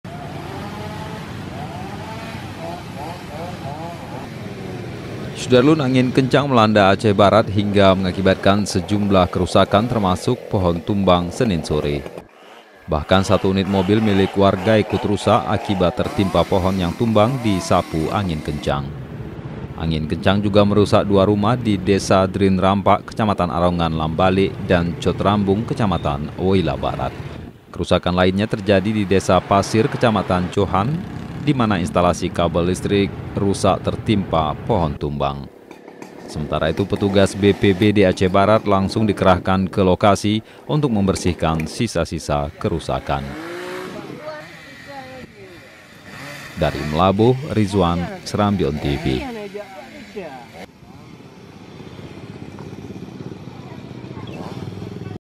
Angin Kencang Timbulkan Sejumlah Kerusakan di Aceh Barat
SERAMBITV.COM, MEULABOH - Angin kencang melanda Aceh Barat hingga mengakibatkan sejumlah kerusakan, termasuk pohon tumbang, Senin (17/6/2019) sore.Bahkan satu unit mobil milik warga ikut rusak akibat tertimpa pohon yang tumbang disapu angin kencang.Angin kencang juga merusak dua rumah di Desa Drien Rampak Kecamatan Arongan Lambalek dan Cot Rambong Kecamatan Woyla Barat.Kerusakan lainnya terjadi di Desa Pasir Kecamatan Johan, dimana instalasi kabel listrik rusak tertimpa pohon tumbang.Sementara itu, petugas BPBD Aceh Barat langsung dikerahkan ke lokasi untuk membersihkan sisa-sisa kerusakan.